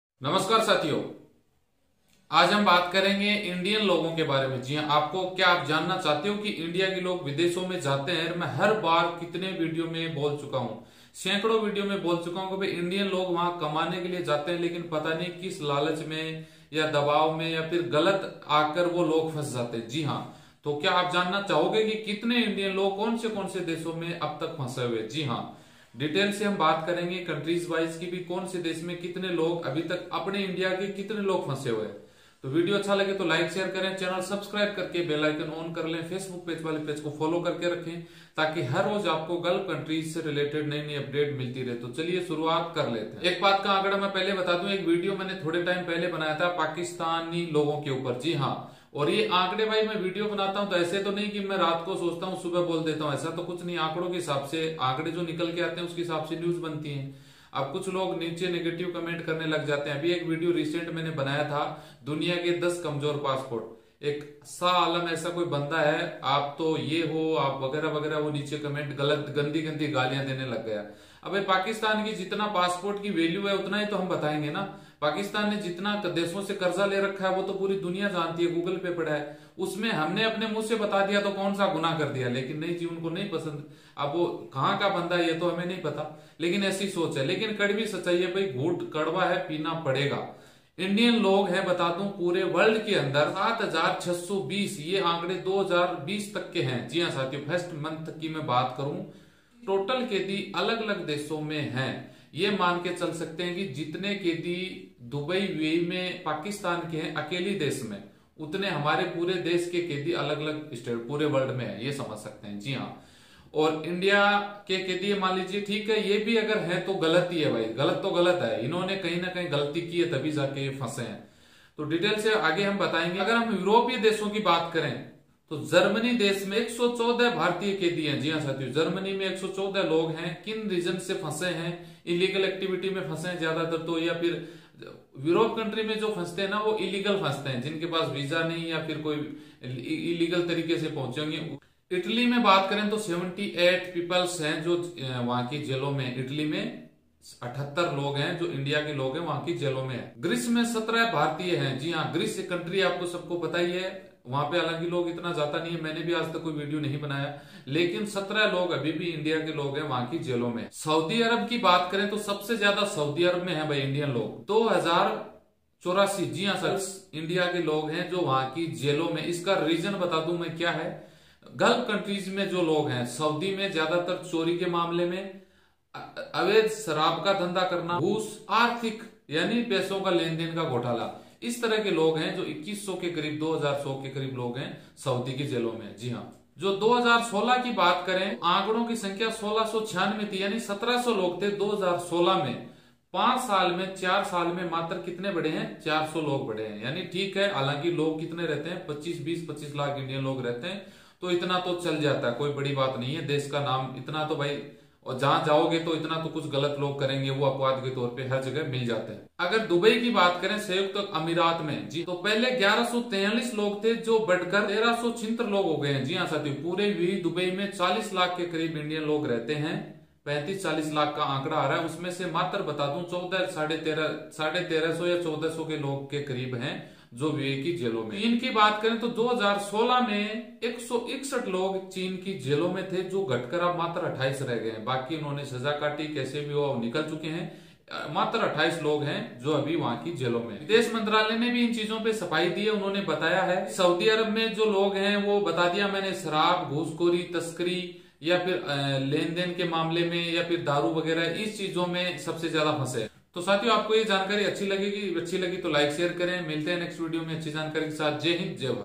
How many people of India are kept in foreign jails. How many in the country.dubai visit visa update
How many people of India are kept in foreign jails.  How many in the country.

IFSC CODE-SBIN0031123
GOOGLE PAY NO.9414503812
PAYTM NO.9414503812
PHONE PAY NO.9414503812
CALL TIME-10AM-5PM
SUNDAY CLOSED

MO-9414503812